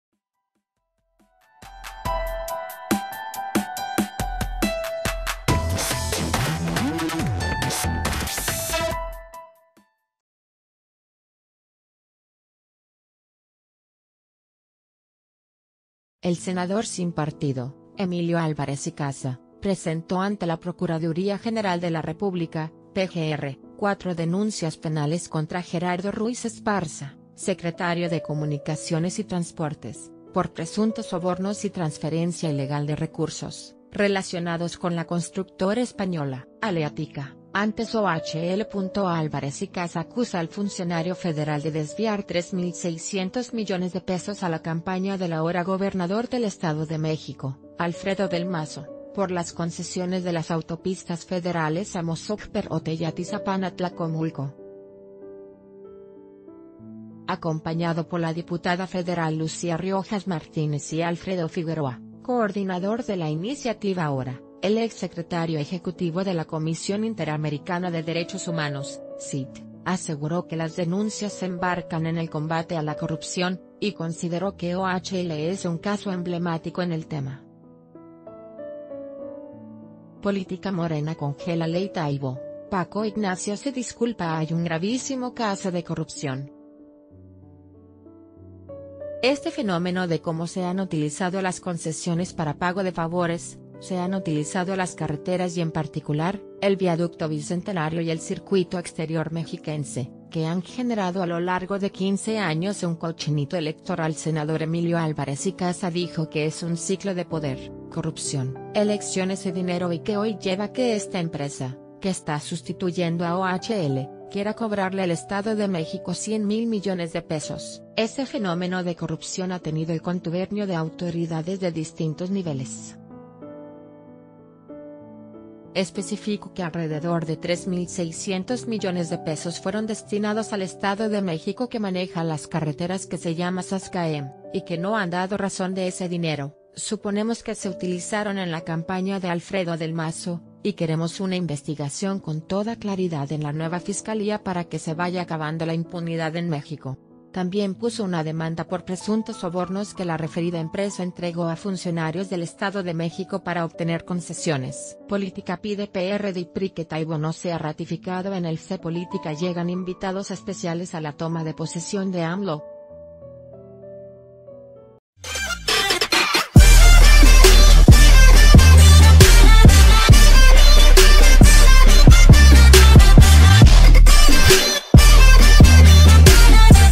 Álvarez Icaza acusa a Ruiz Esparza de millonarios desvíos a OHL
El senador sin partido, Emilio Álvarez Icaza, presentó ante la Procuraduría General de la República (PGR) cuatro denuncias penales contra Gerardo Ruiz Esparza, secretario de Comunicaciones y Transportes, por presuntos sobornos y transferencia ilegal de recursos, relacionados con la constructora espa...